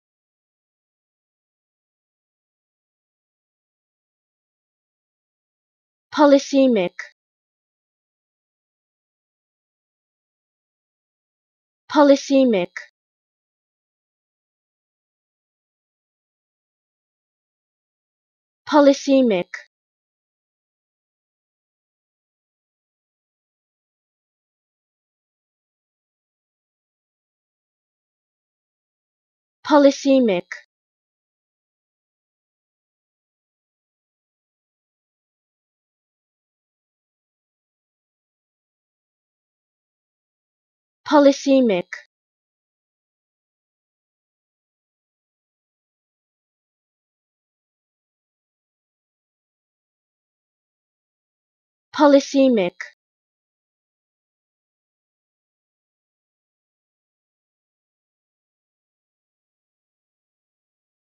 How To Say Polysemic
Learn how to say Polysemic with EmmaSaying free pronunciation tutorials.